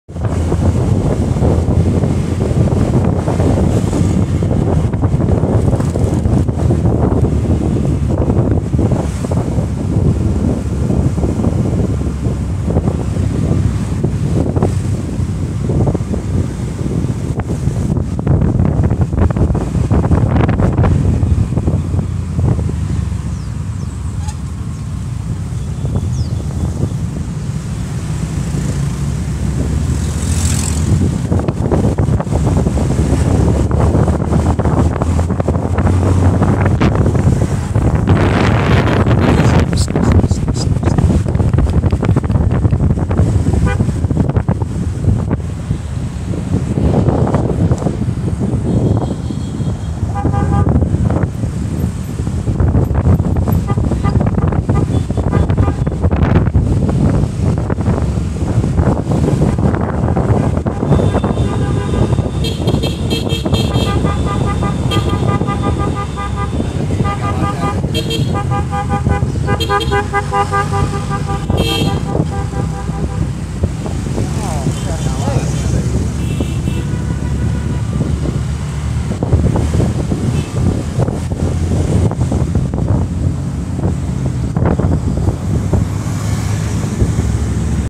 GOJEK Surabaya Ziarah 5 Wali
Driver GOJEK Surabaya, lagi perjalanan (konvoi) Ziarah 5 Wali, bukan hanya duniawi yang mereka cari, tapi kepuasaan hati bisa mendoakan dan Ziarah 5 Wali !!!